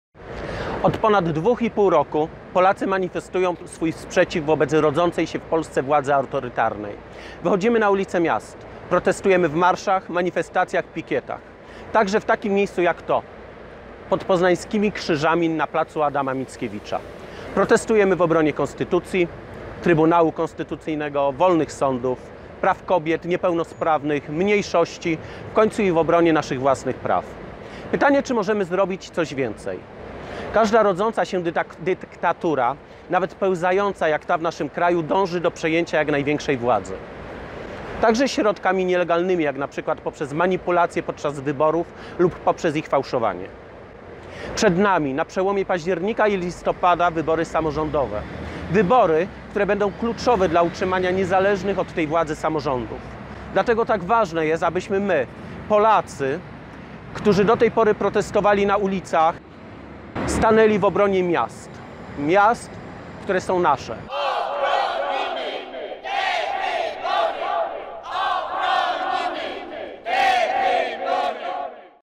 Sławomir Majdański
Niezależnie od tego czy mieszkasz w mieście, czy na wsi, spokój twojej Małej Ojczyzny jest zagrożony. Ktoś chce wprowadzić do Twego domu swoje porządki. Zgodzisz się na to?